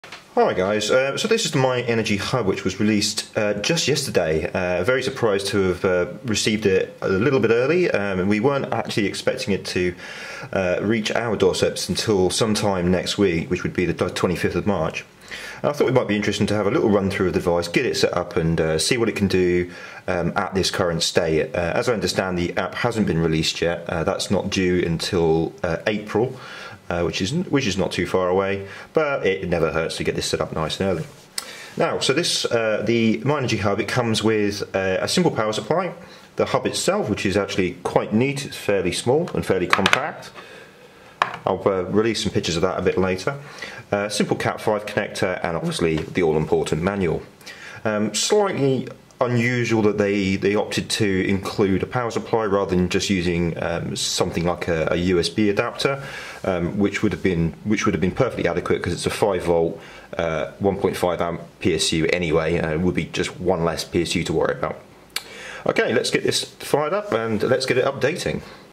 Unboxing the myEnergi Hub (Part 1)
As one of the first myEnergi Hubs to get into a consumers hand I thought it'd be useful to give a little introduce and go through the installation process.

The Hub is a device that links together all your myEnergi devices allowing remote monitor and control via the still to be released app.